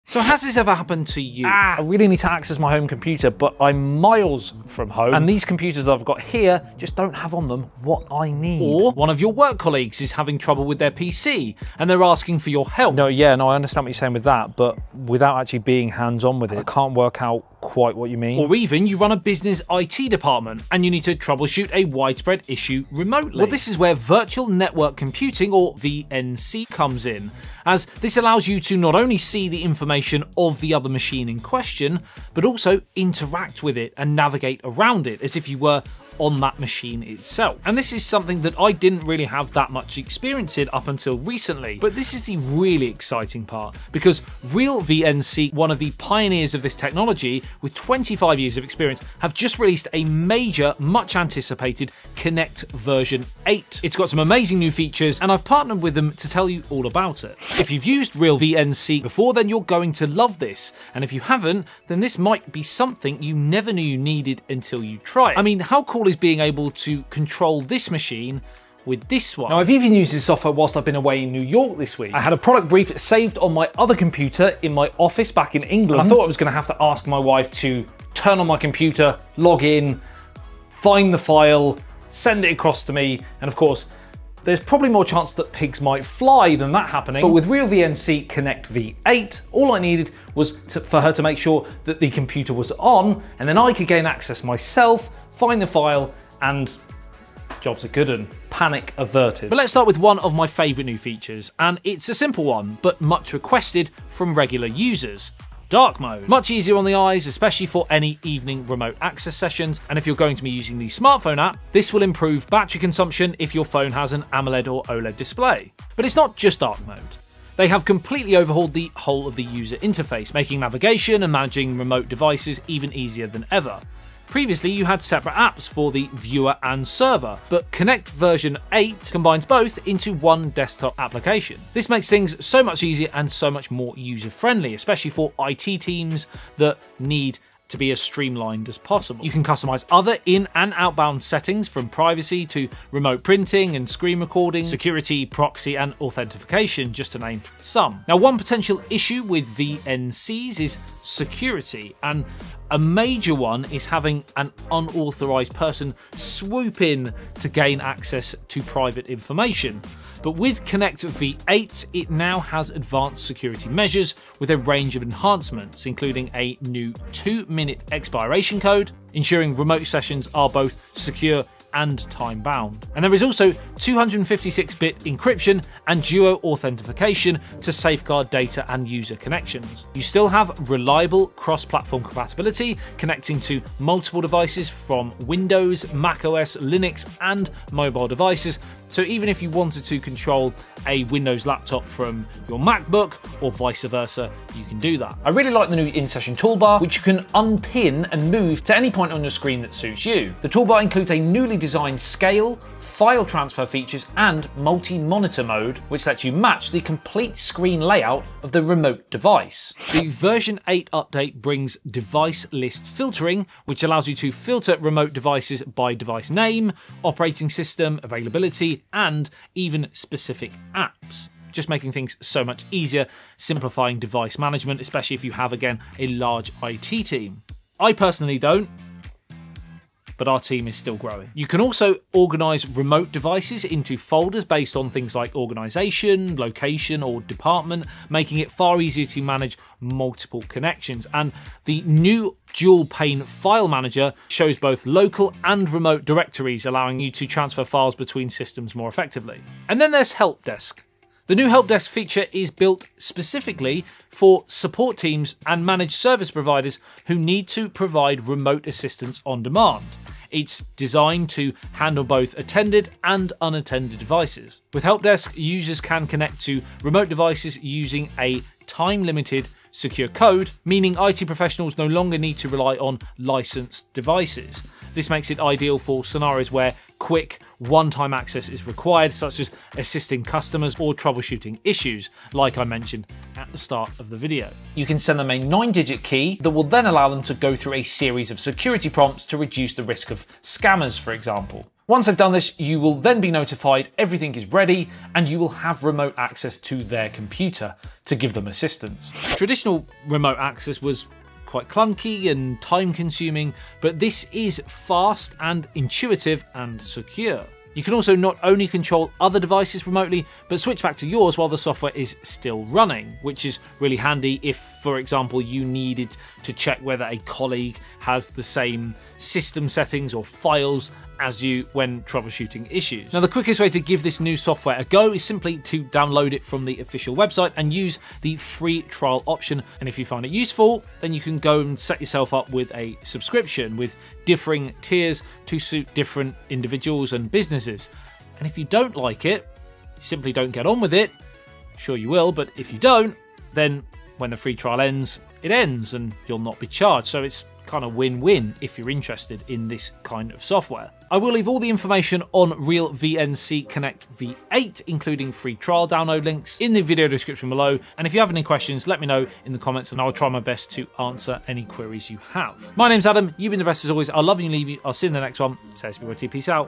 Connect to ANY computer from ANYWHERE! - (RealVNC Connect V8)
Need to access your home or work PC from anywhere? Meet REALVNC Connect V8 — the major new update packed with powerful features! With a sleek Dark Mode, a fully overhauled user interface, stronger security with 256-bit encryption and two-minute guest codes, plus new tools like HelpDesk for IT teams, remote access has never been faster or safer. Connect across Windows, Mac, Linux, and mobile devices, transfer files effortlessly, and manage large device lists with ease.

This video is a walkthrough tutorial on all the new upgrade features.